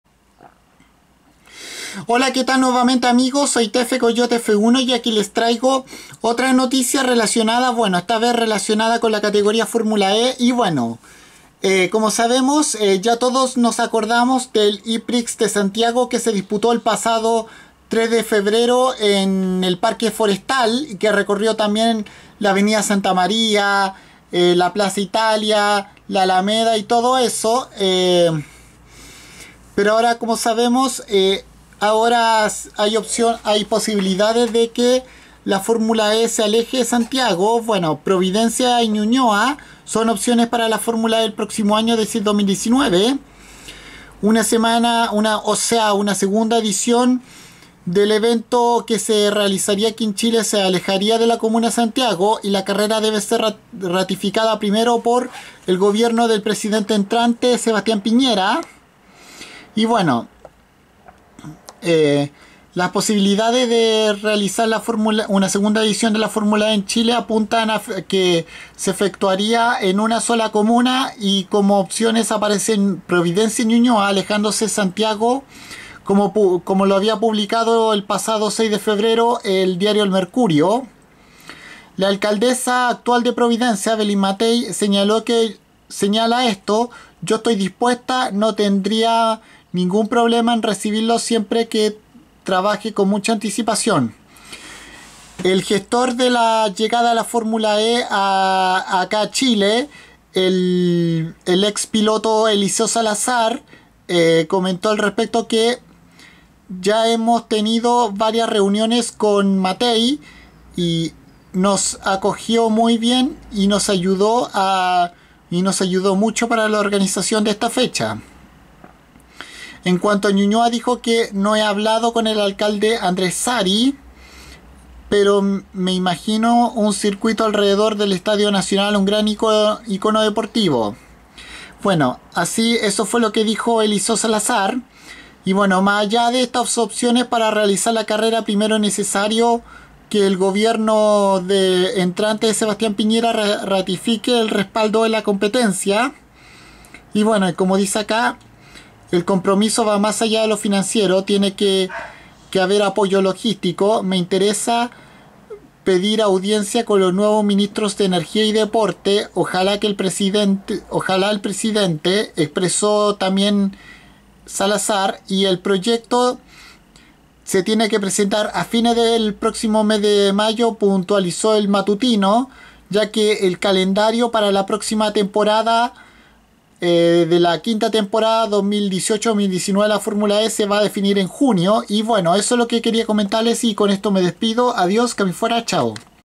Ñuñoa y Providencia opciones para la Formula E en Santiago en 2019
Después de la reciente primera edición del eprix de Santiago de Chile ahora las comunas de Ñuñoa y Providencia se presentan como opciones para la próxima edición de este evento en 2019